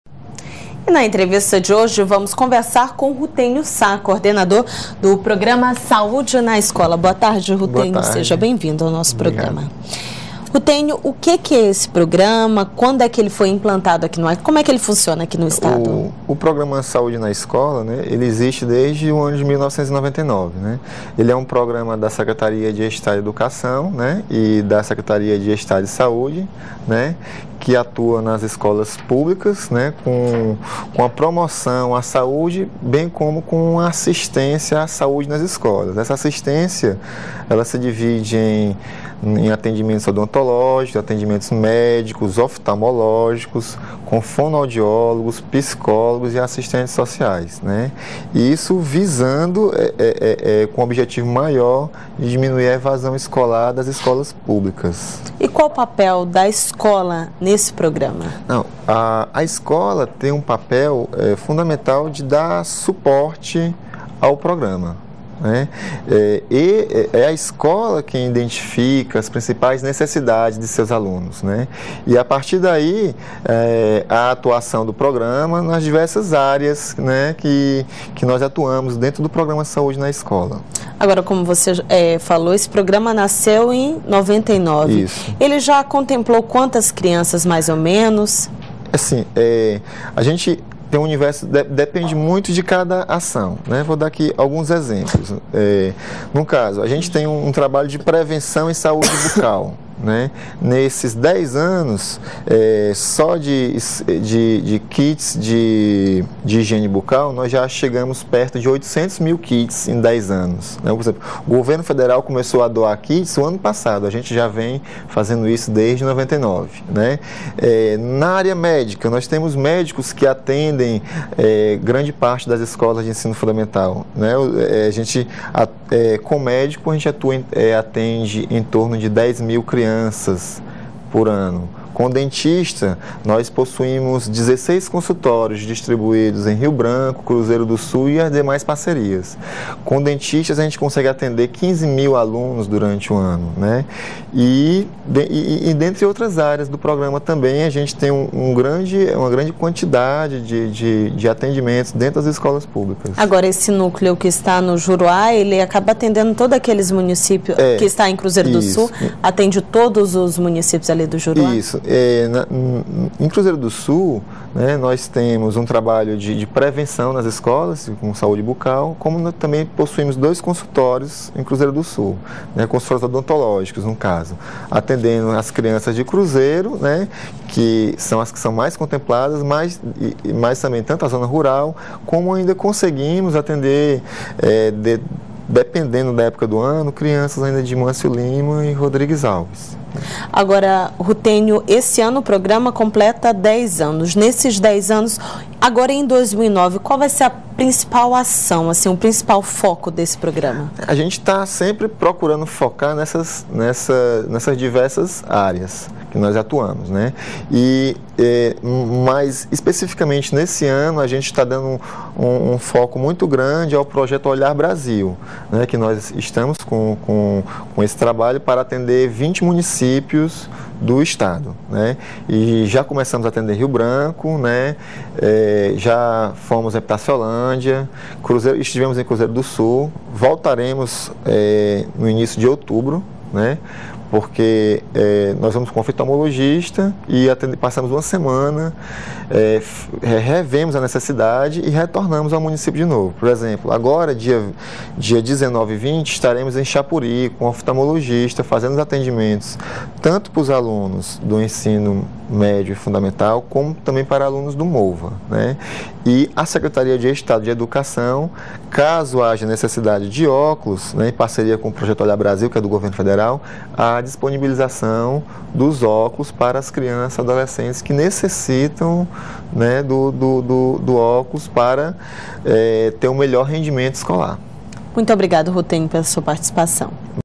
Entrevista com o coordenador do programa Saúde na Escola Rutênio Sá
Conheça como atua o programa da Secretaria de Estado de Educação “Saúde na Escola”. O coordenador do projeto Rutênio Sá explica como funciona.